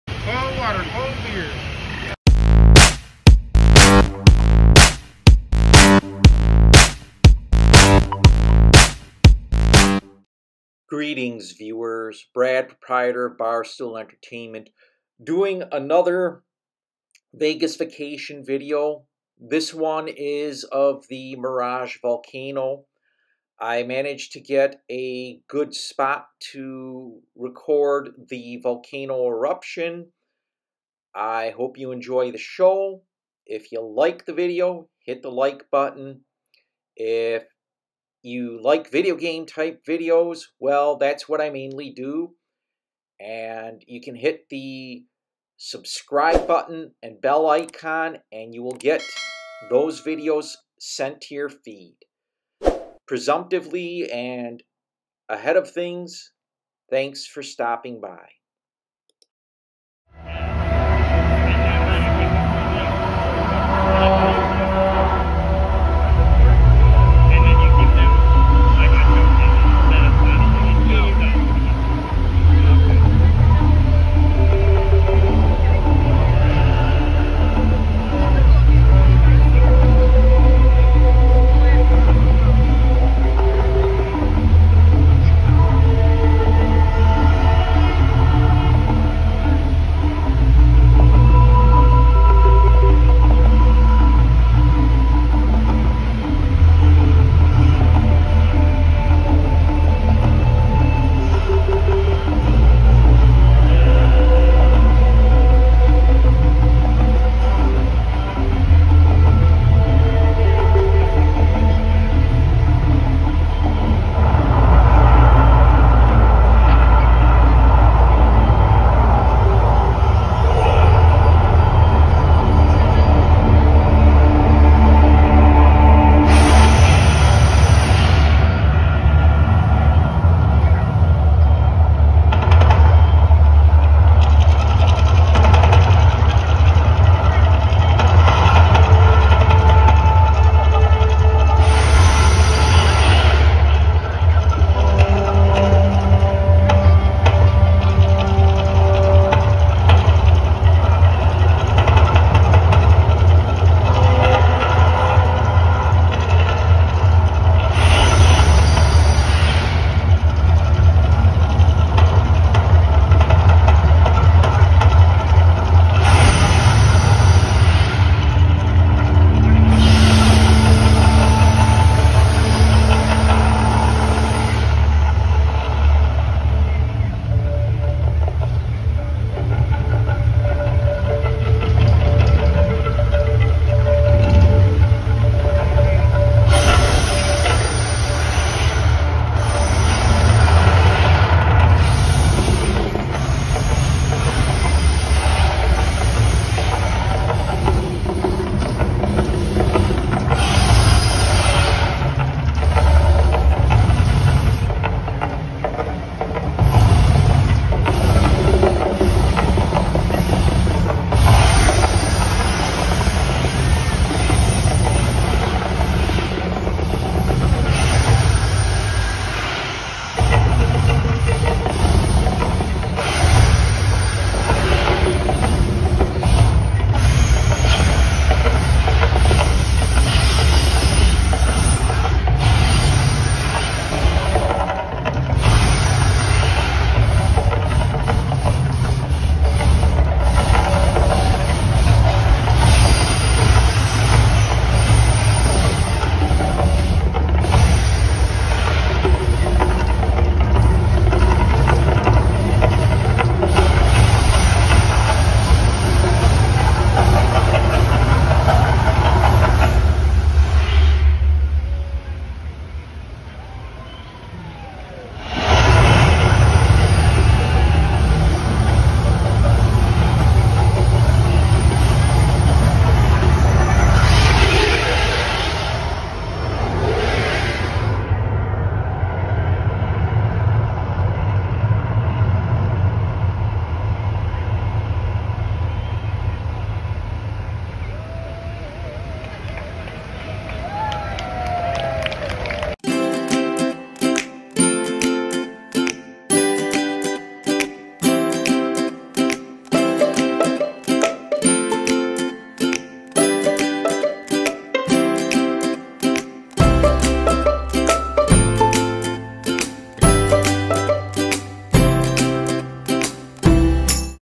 Mirage Las Vegas Volcano
I got a decent spot to record the hourly eruption of the Mirage Casino's Volcano.